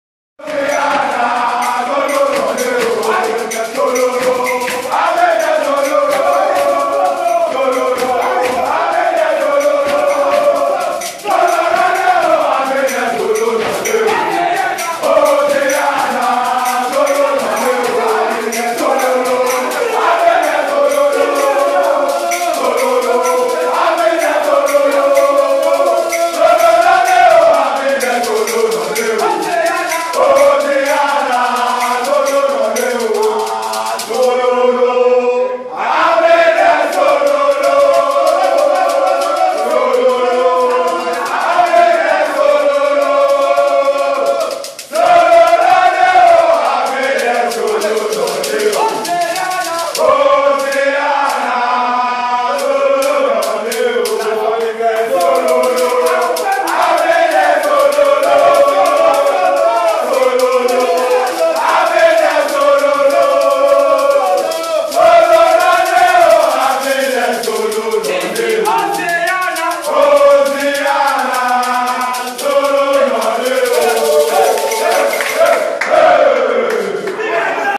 Ghana players arrive in stadium in charged mood ahead of AFCON 2015 match against Guinea
Ghana players arrive in stadium in charged mood ahead oftheir 2015 Africa Cup of Nations quarter-final match against Guinea in Malabo.